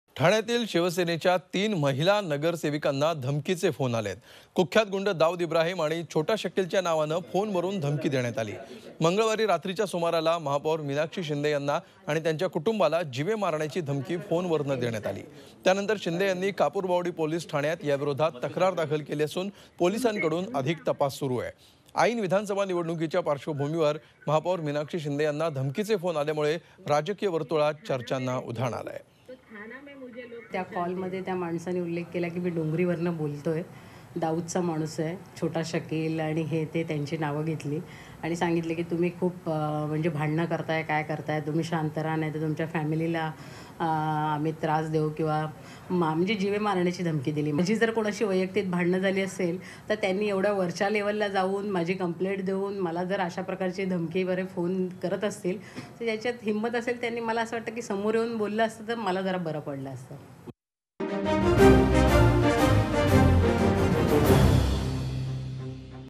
ठाणे | महापौर मिनाक्षी शिंदे यांना दाऊदच्या नावाने फोन
Threats To Thane  mayor Meenakshi shinde  in the name of Dawood  |18 Sep 2019